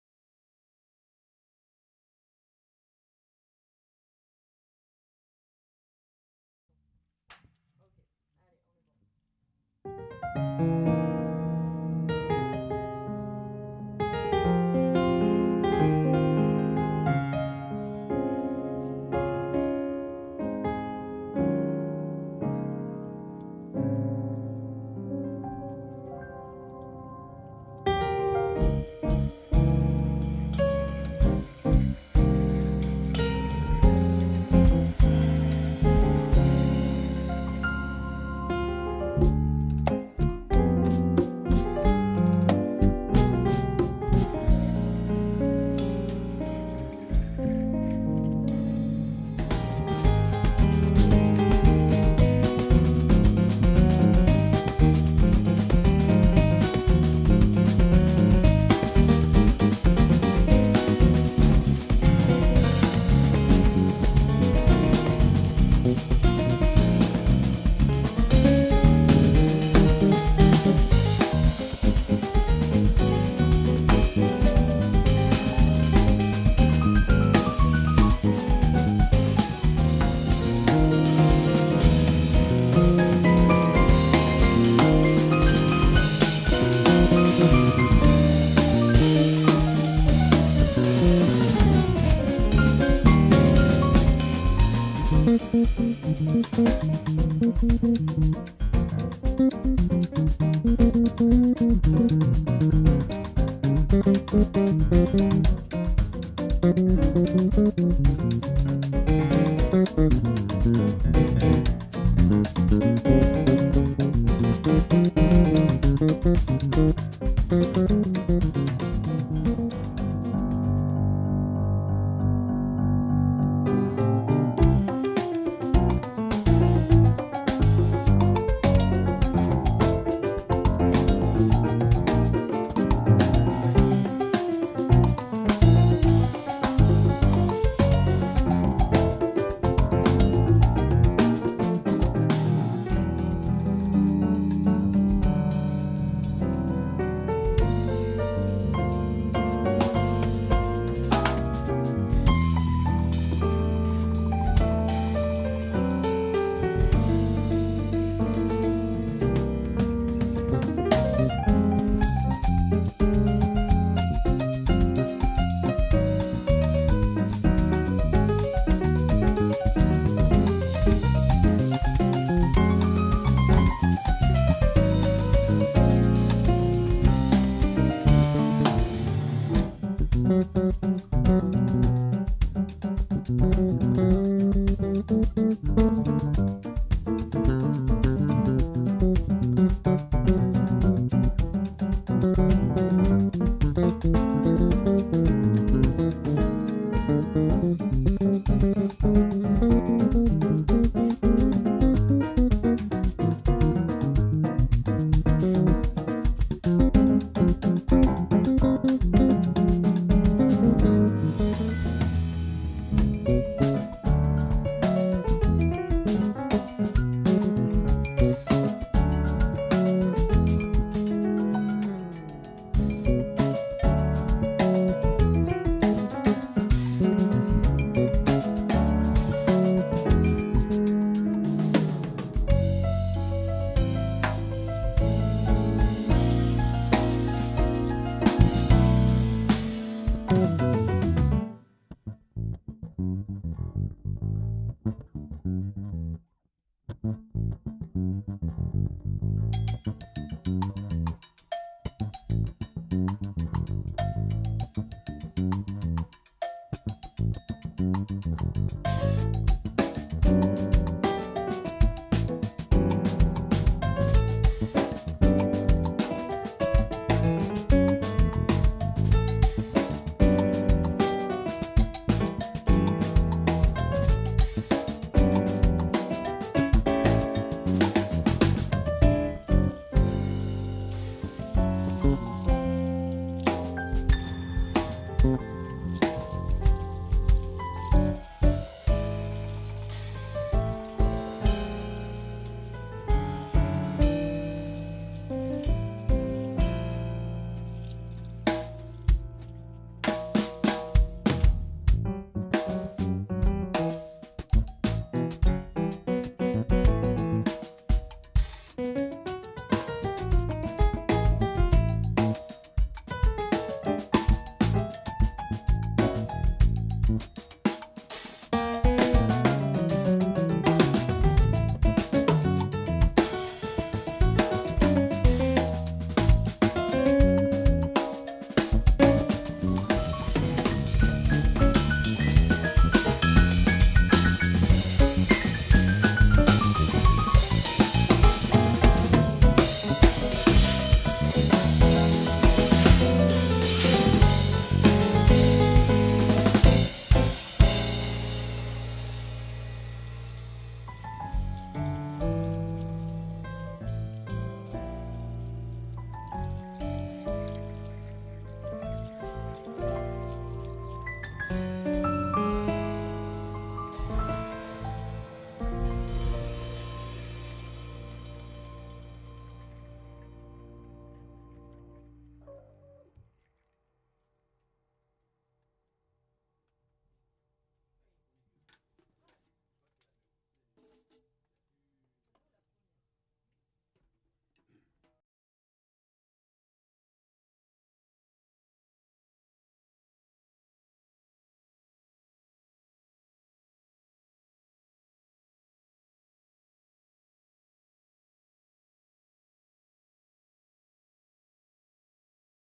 Teaser groupe Ginga - Afrolatin jazz
Alain Vernerey - Piano
Rémi Alessandra - Bass
Claude Santelli - batterie
Jorge Santos - Percussions